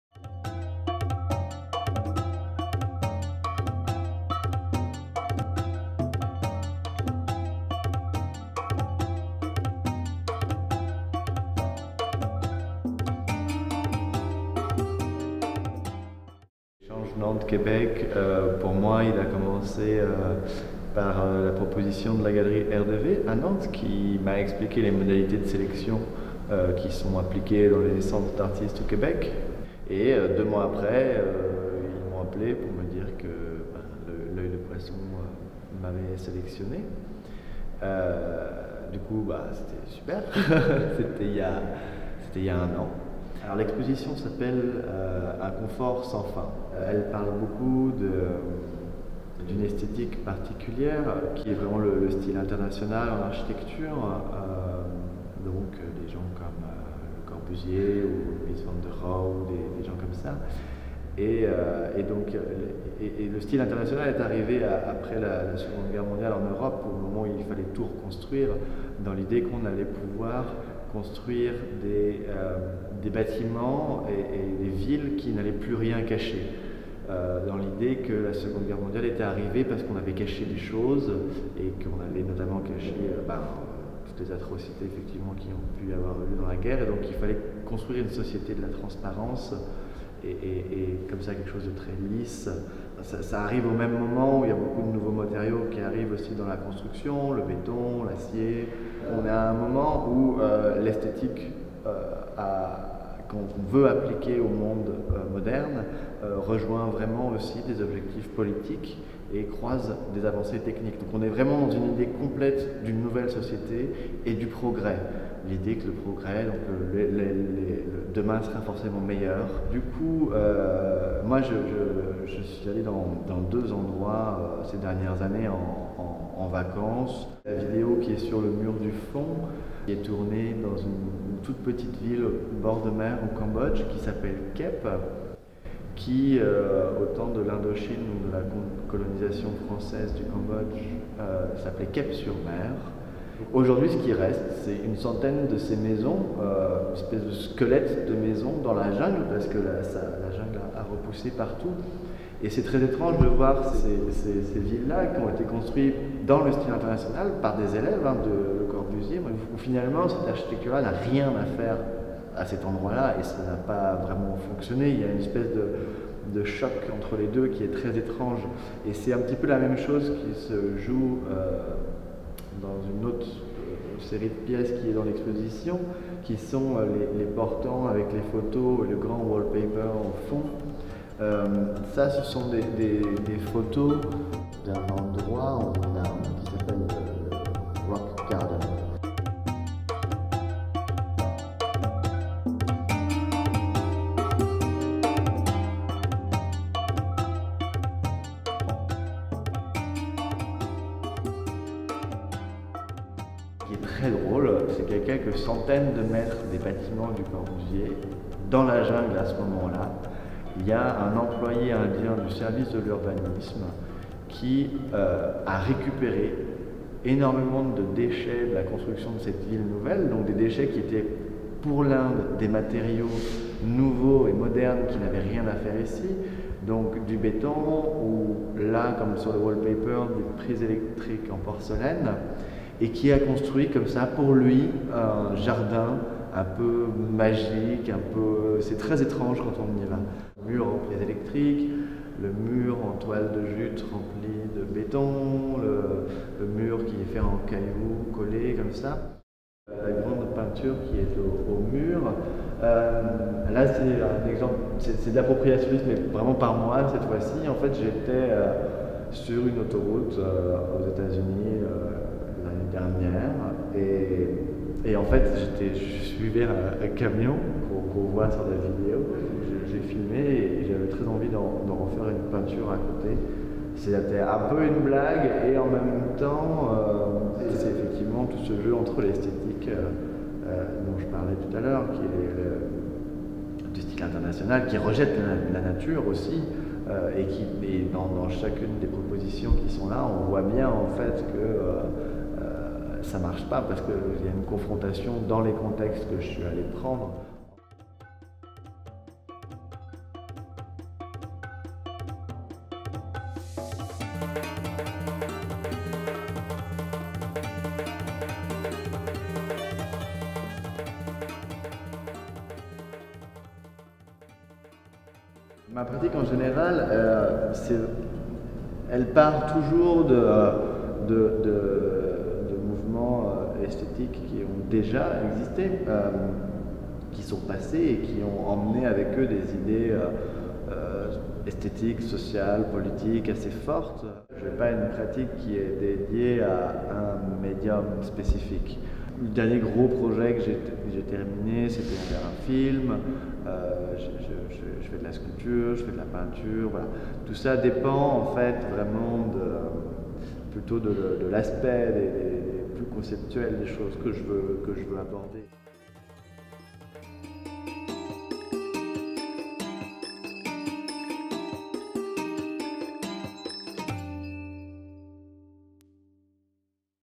Hoël Duret - Un confort sans fin
Après avoir présenté le travail de Jeffrey Poirier à la Galerie RDV à Nantes au printemps dernier, l'Œil de Poisson est heureux de d'accueillir le jeune artiste nantais Hoël Duret à l’occasion de l’échange Québec-Nantes initié par Manif d’art.

Le travail d'Hoël Duret peut être vu comme une archéologie critique des formes modernes. Du design à l'architecture en passant par les arts visuels, l'artiste traque et
débusque les symptômes d'un passé glorieux qu'il réemploie ensuite dans ses sculptures, vidéos et peintures. Comme un carnet de voyage, Un confort sans fin présente des fragments du modernisme européen dans des lieux tantôt aride, tantôt exotique. À Chandigarh en Inde, Hoël Duret s'est intéressé à Rock Gardens situé à quelques pas des architectures de Le Corbusier. Issu d'une réappropriation artistique, cet environnement conçu à partir de résidus de matériaux se pose comme une synthèse fascinante des avancées techniques modernes mises à mal par une récupération typiquement tiers-mondiste. Dans une même logique, c'est en suivant un camion dans un canyon de l'Ouest américain que Duret voit dans son chargement une peinture géométrique; un peu comme si l'abstraction jaillissait subitement pour arpenter ce paysage connoté. Puis, à Kep au Cambodge, il filme les ruines de villas modernistes françaises saccagées à la suite de l'arrivée au pouvoir du régime communiste. Dans la jungle lumineuse, ces architectures se posent telles les stèles d'un passé lointain.

Né en 1988, Hoël Duret vit et travaille à Nantes et Paris. Il détient un Diplôme National d’Expression Plastique Art (DNSEP) de l’École supérieure des beaux-arts de Nantes. Son travail a été présenté lors d’expositions individuelles et collectives en France, en Espagne, aux États-Unis, en Chine et en Corée du Sud. En 2015, il a reçu le Prix Yishu 8 et le prix des Arts visuels de la Ville de Nantes. De plus, il sera en résidence au Pavillon Neuflize OBC, laboratoire de création du Palais de Tokyo à Paris et prendra part à une exposition collective au Séoul Art Museum au cours de 2016.